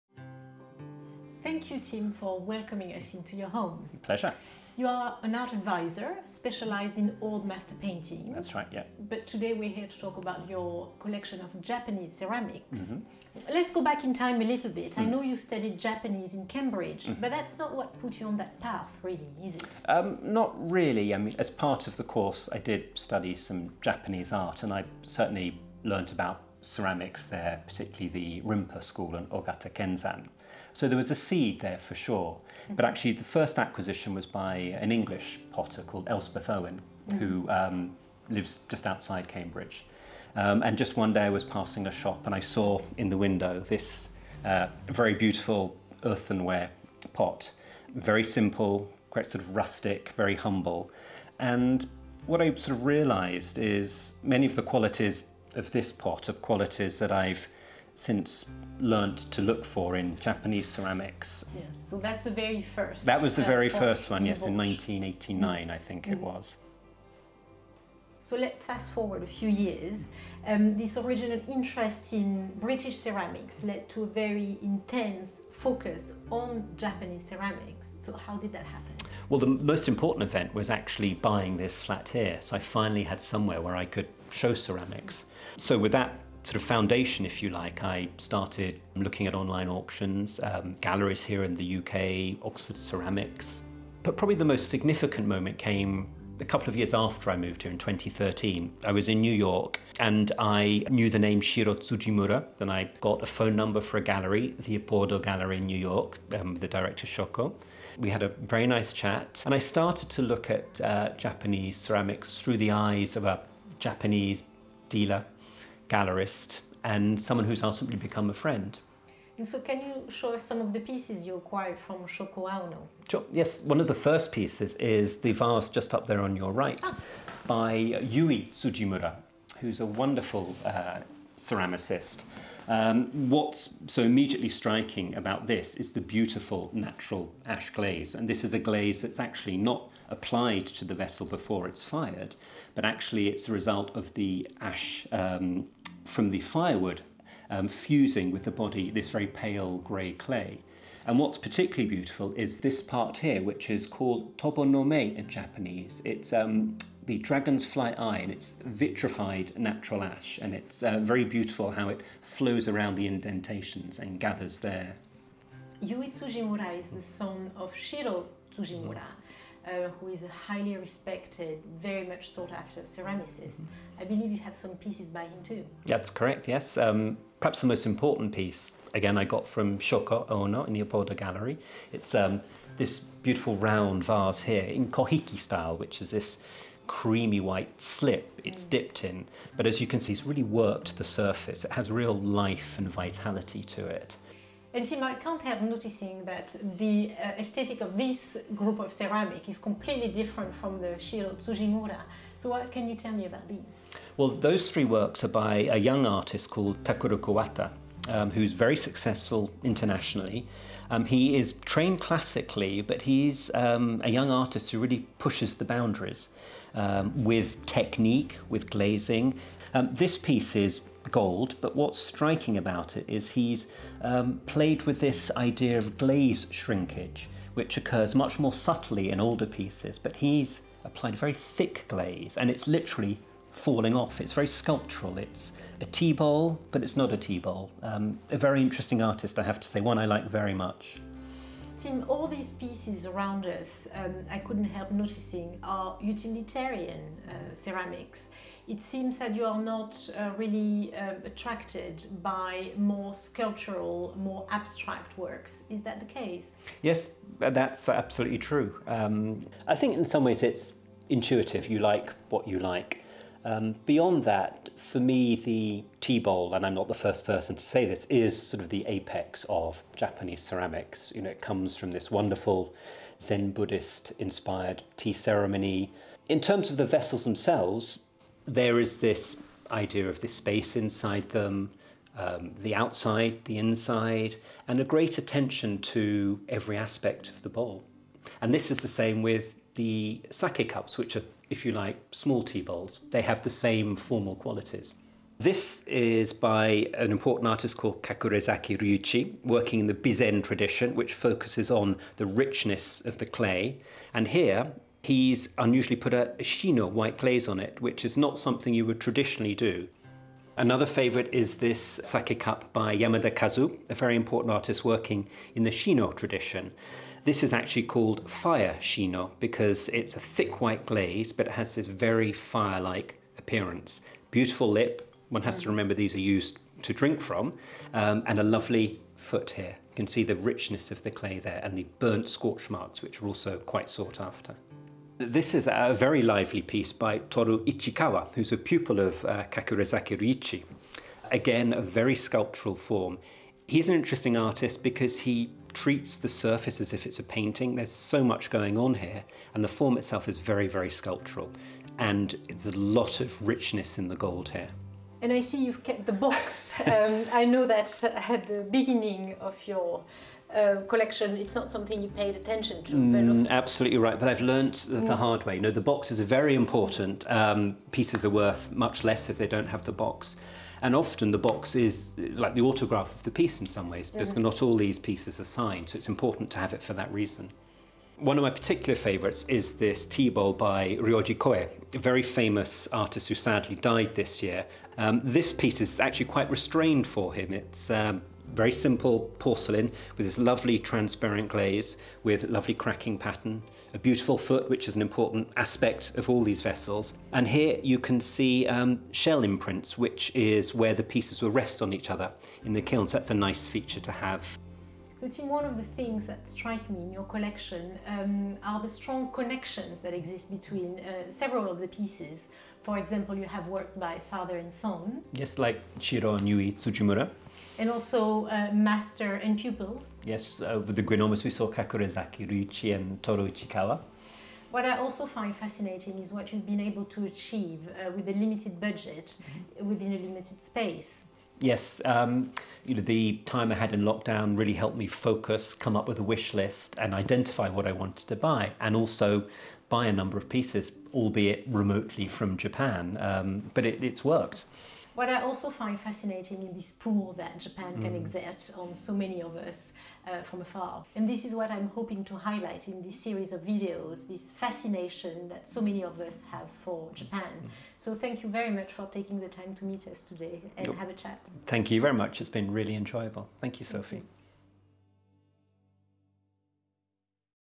Encounters with Japan: Collecting Japanese ceramics
Tim Warner-Johnson has been building an interesting collection of Japanese ceramics over the last few years, with limited budget and without going to Japan. His inspiring story shows what can be achieved with passion and focus. He discusses some of his favourite pieces with Sophie.

Tim and Sophie looked at works by the following artists (in order of appearance on the video):

Elspeth Owen (b. 1938)
The very first piece of ceramic acquired by Tim was by Elspeth Owen, a British studio potter active in Cambridgeshire.

Yui Tsujimura (b. 1975)
Living and working in Nara, he is the first son of famed potter Shiro Tsujimura. His signature ash-glazed pieces are fired in a kiln he built for himself in 2000.

Shiro Tsujimura (b. 1947)
One of the leading ceramic artists in Japan today, he lives in the mountains outside of Nara. Self-taught, he creates powerful functional vessels in a wide-ranging repertoire. He shows a particular fondness for a technique known as kohiki in which a vessel is dipped, either partially or in its entirety, in white slip.

Takuro Kuwata (b. 1981)
Living in Gifu Prefecture, Kuwata is best known for his contemporary take on the venerable Japanese tea bowl. Breaking away from traditional aesthetic, he adopts a strong palette, glistening glazes and fractured forms.

Ryuichi Kakurezaki (b. 1950)
Born in Nagasaki, he now lives in Bizen where he has developed his own original style. His sharply cut forms and three-legged vessels have become his trademark.

Kazu Yamada (b. 1954)
Hailing from a family of ceramicists, he produces mostly Mino wares and is especially admired for his mastery of various shino glazes, including the striking ‘dancing fire’ glaze.

Toru Ichikawa (b. 1973)
An apprentice of Ryuichi Kakurezaki, Ichikawa established his own kiln in Bizen in 2015. He creates beguiling, dynamic and colourful ceramics that have earned him a fast-growing popularity.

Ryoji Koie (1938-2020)
One of the most influential figures in the world of Japanese ceramics, Koie was a playful and provocative artist. His very diverse output shows his taste for experimentation and his inclination for breaking the rules.